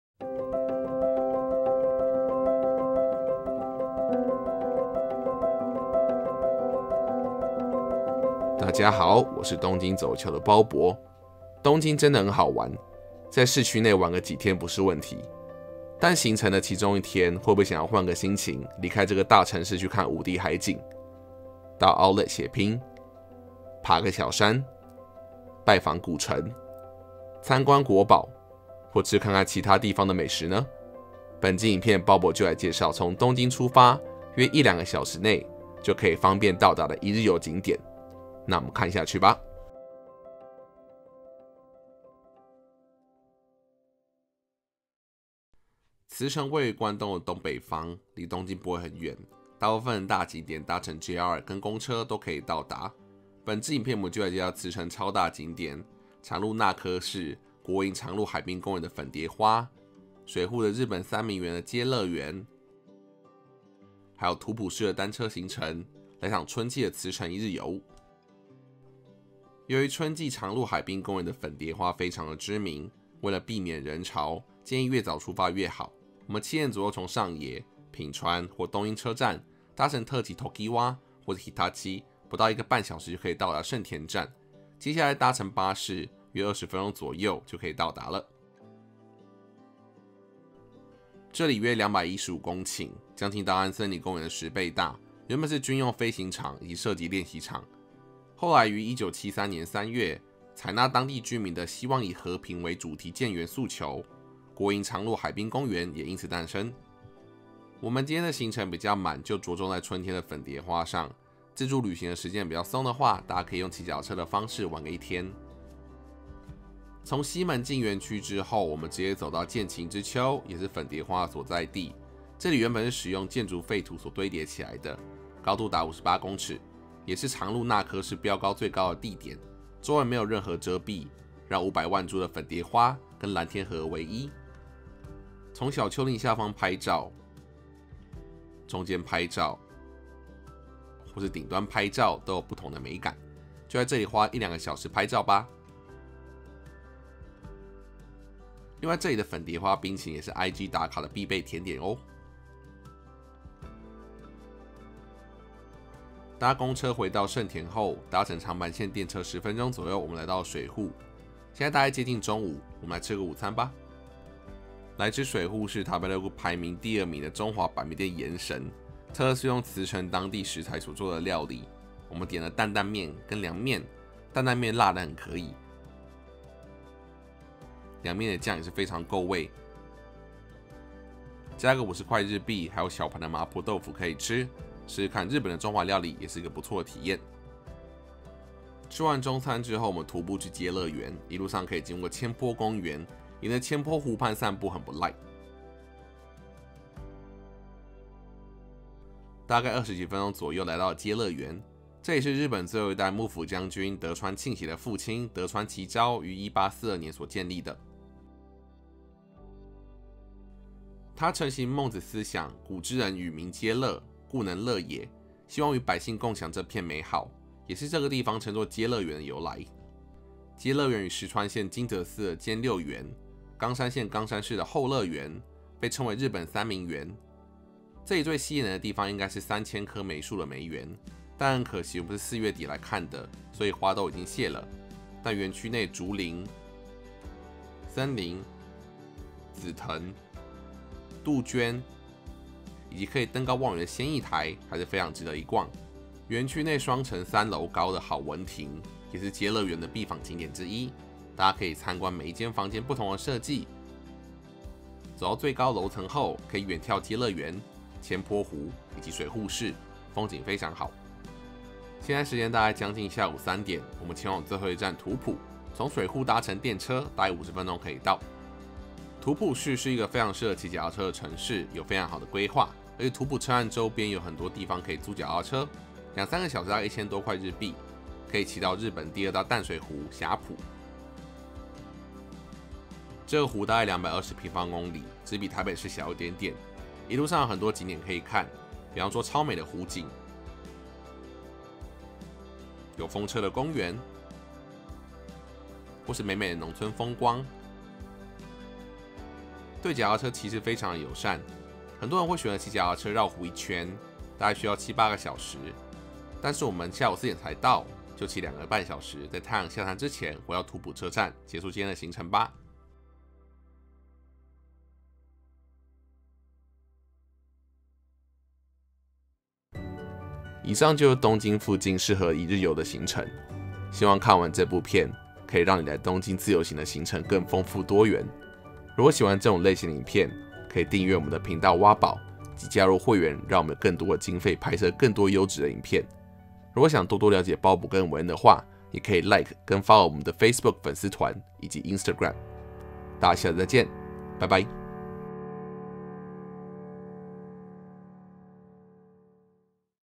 大片藍色花海、日本三大名園、騎腳踏車環湖｜東京近郊一日遊
茨城位於關東的東北方，離東京不會很遠。大部分的大景點搭乘JR跟公車都可以到達。本支影片我們就來介紹茨城超大景點 常陸那珂市 國營常陸海濱公園的 粉蝶花，水戶的日本三名園的 偕樂園，還有 土浦市 的單車行程。來場春季的 茨城一日遊。